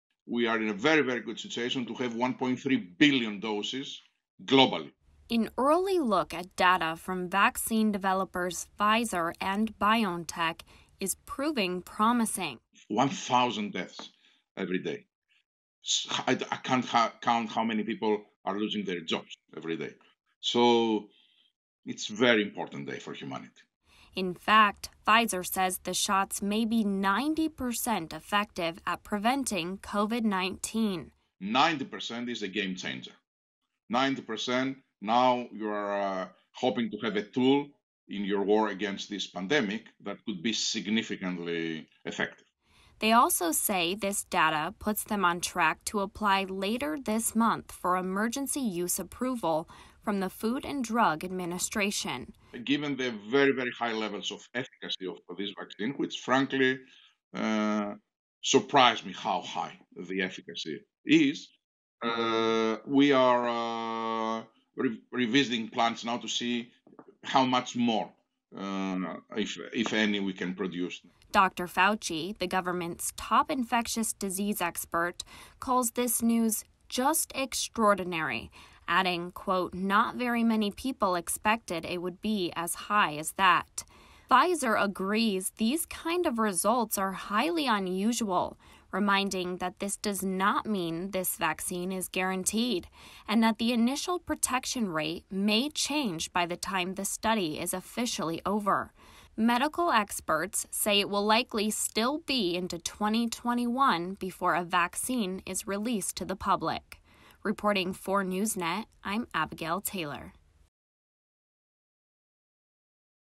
Pfizer: COVID-19 vaccine looking 90% effective
Pfizer said Monday that an early peek at data on its coronavirus vaccine suggests the shots may be a surprisingly robust 90% effective at preventing COVID-19, putting the company on track to apply later this month for emergency-use approval from the Food and Drug Administration.